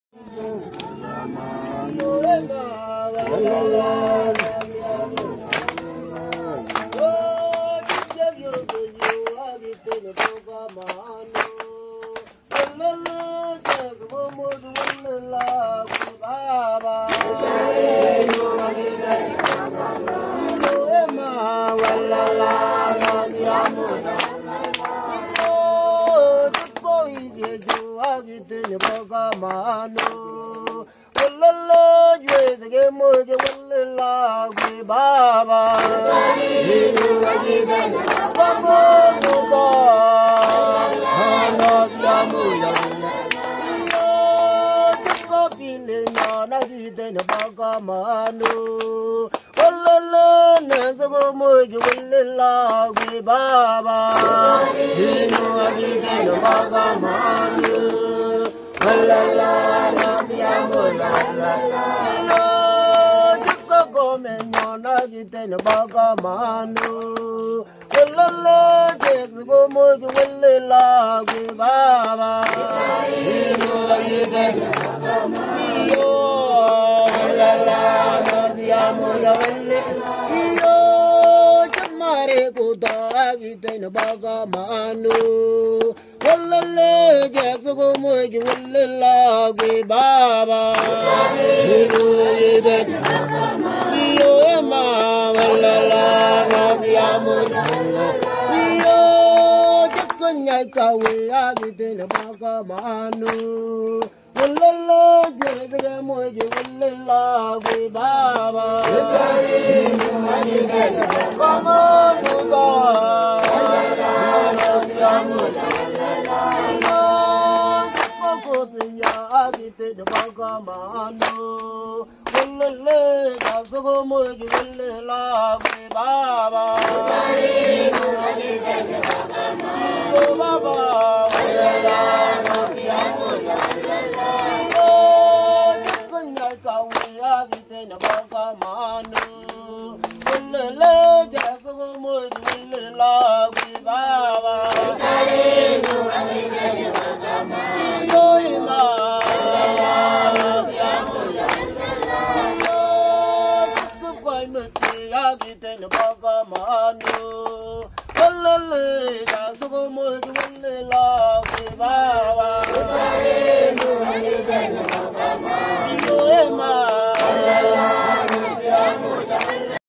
Hymn Singing in Swahili, Kerio Valley, Kenya
Every Sunday these Kenyan brothers and sisters meet under the tree to have a worship service. Here they are singing a hymn in Swahili.